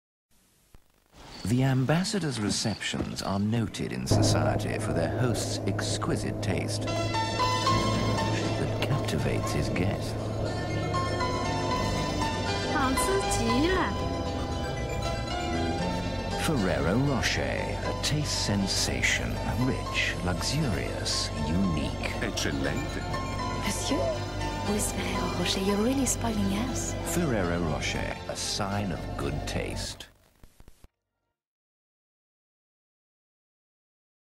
Monsieur, wiz Ferrero Rocher you're really spoiling us
A beautiful love story blossoms out on the road at some traffic lights. I know right... Such a cliche.